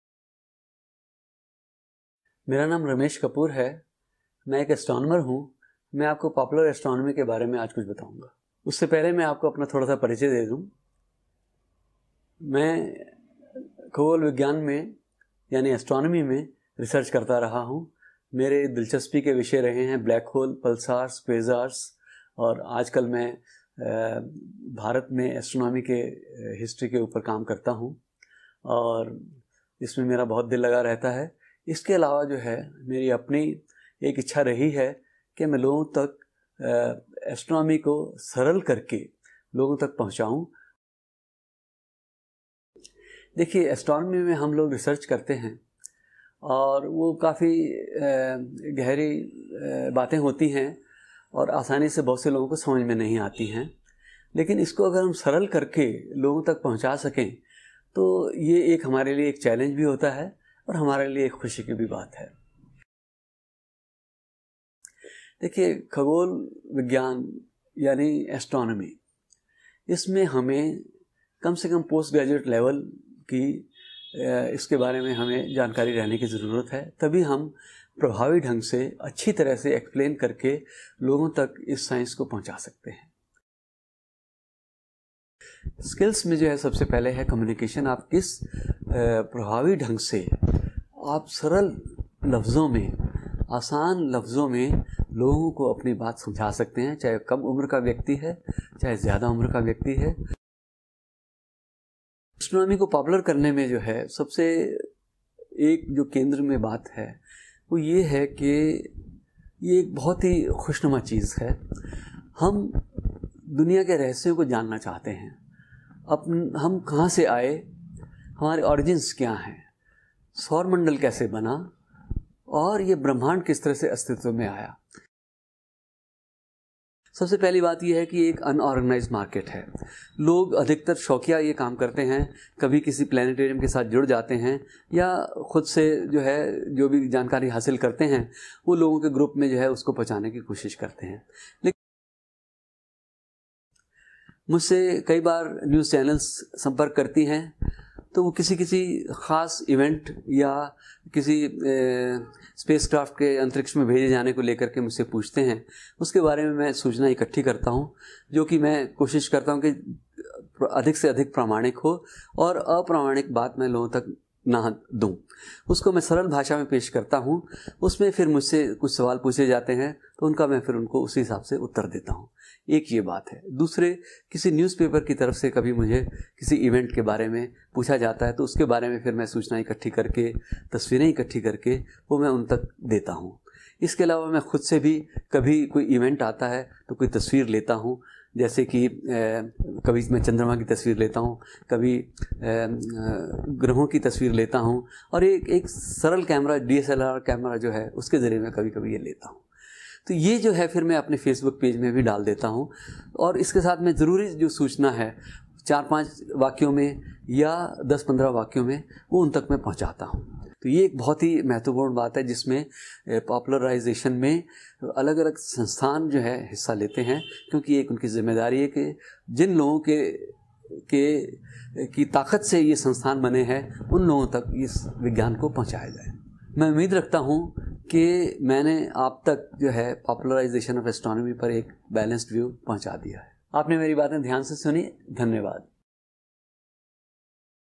LifePage Career Talk on Astronomy Popularization
By R C Kapoor
[Astronomer in Various Assignments]

What is Astronomy Popularization?
R C Kapoor: "Astronomy is such a fascinating subject that attracts not only students but also layman and amateurs. Curiosity to understand the subjects is very common in all age groups. Historically, Astronomy is one of the oldest sciences concerned with the study of planets, stars, galaxies & other celestial objects and phenomena. From time immemorial the night sky has fascinated many cultures in the past including the Indians, Babylonians, Egyptians, Chinese & the Greeks and it continues to fascinate us even today."

How I got into Astronomy Popularization?
R C Kapoor: "My inclination towards astronomy led to my graduation and then post graduation in Astrophysics. In 1971, I started my career and joined Observatory (now Aryabhatta Research Institute of Observational Sciences, ARIES) at Nainital in observational astronomy. Then, in 1974 until 2010, I was with the Indian Institute of Astrophysics (IIA), Bangalore where I worked on various topics in relativistic astrophysics - black holes, white holes, quasars and pulsars etc. His association with the institution continues. In 1980, I completed my Ph D from Agra University. I actively participate in popularization of astronomy and have also published research papers in peer-reviewed international journals and presented papers in national and international conferences. I have also published on the physical-chemical nature of bhasmas in the Indian Systems of Medicine."

(Astronomy Popularization, R.C Kapoor, Indian Institute of Astrophysics, IIA, Astronomy, Cosmology, Space Science, Stars, Planets, Galaxies, Nebulae, Astrophysics)